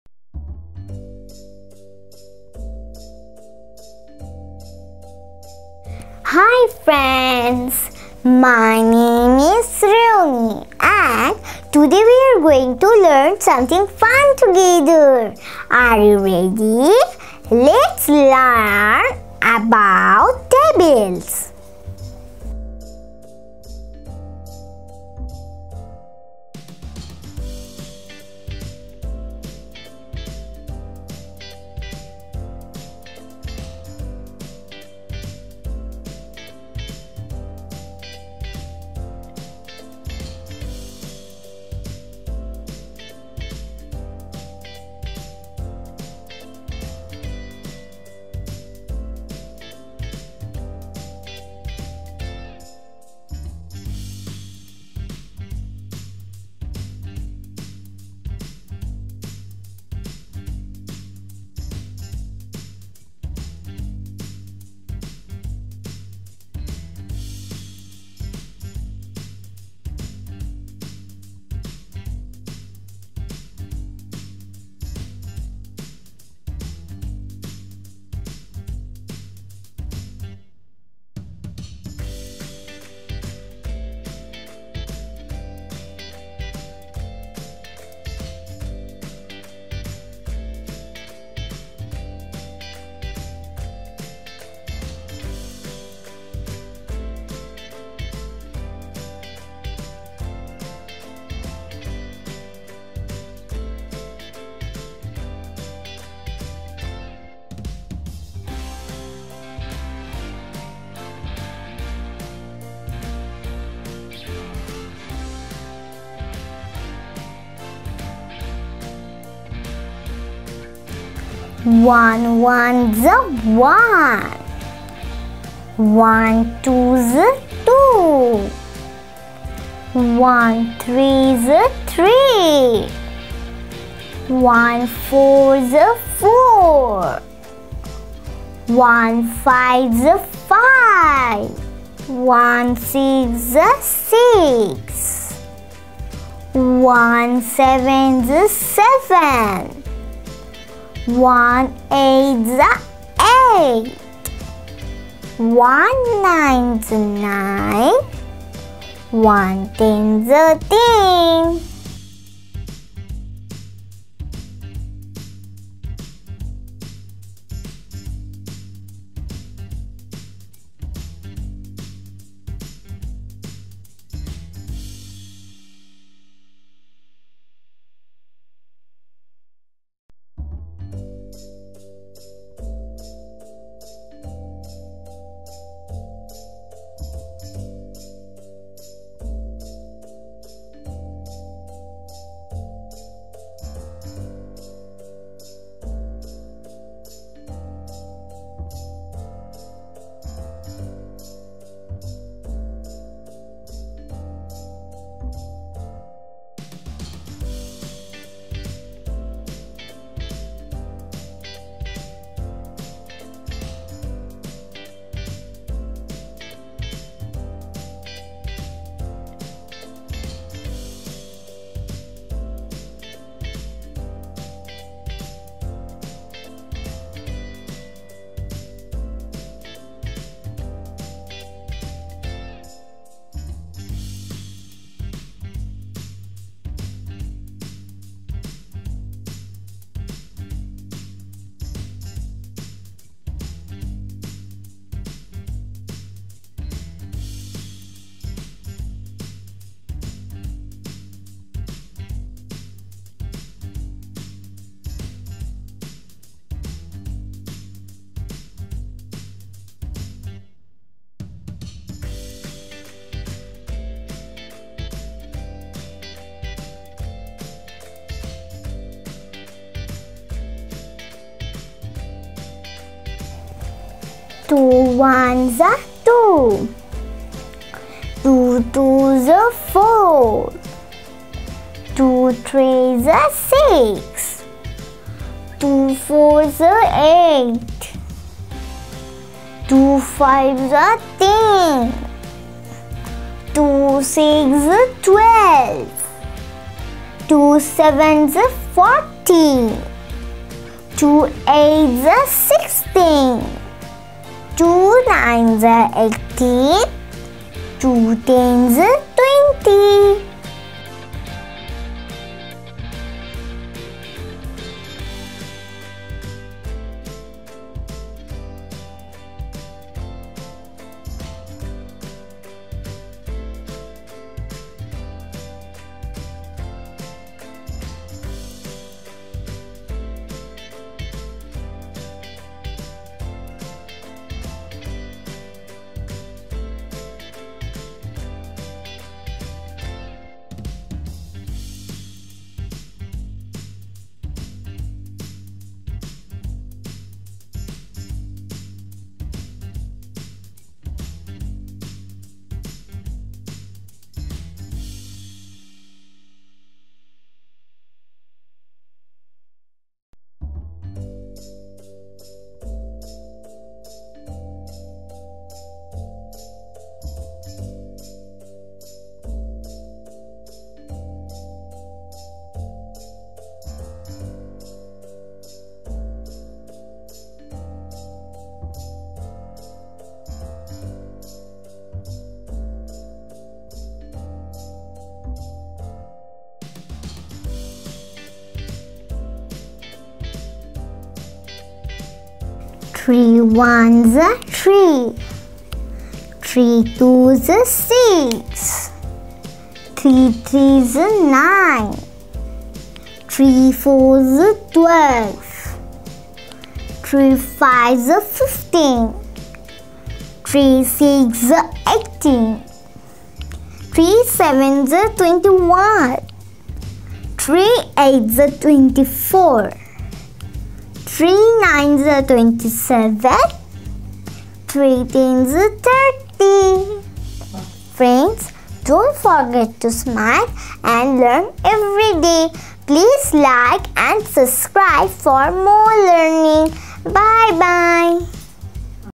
Learn Multiplication Tables 1 to 3 | Easy Math Tables for Kids
Let your kids learn multiplication tables from 1 to 3 in a fun and easy way! This video is perfect for preschoolers, kindergarten, and early primary students. Help them build a strong foundation in math through simple repetition and clear visuals.

🔹 Tables covered: 1, 2, 3
🔹 Kid-friendly format
🔹 Perfect for age 4–7
🔹 Great for homeschooling and school revision